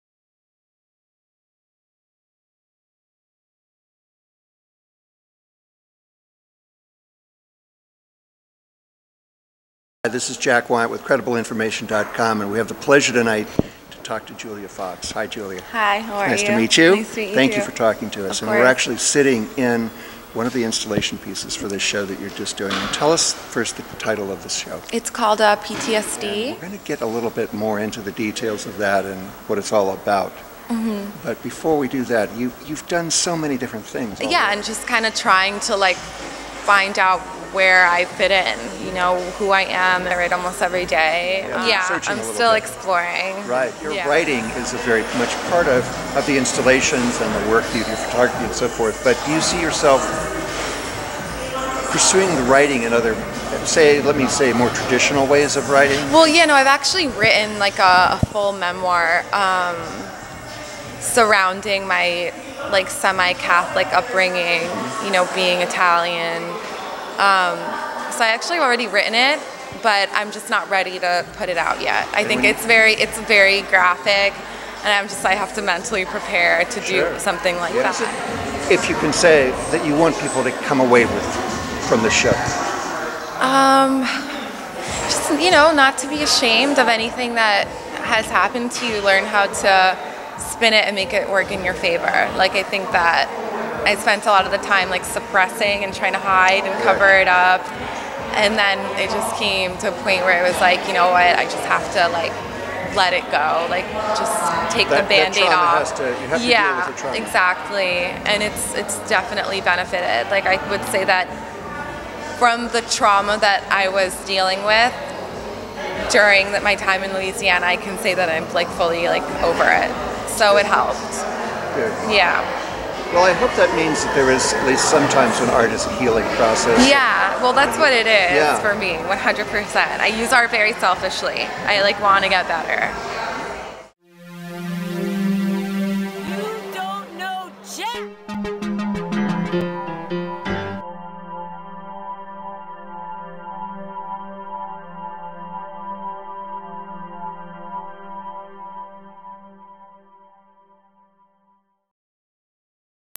PTSD Interview with artist Julia Fox © 2016 credibleinformation.com
Jack Wiant Interviews artist Julia Fox
Discussing Her latest work and the impact of PTSD
06/16/2016 in DTLA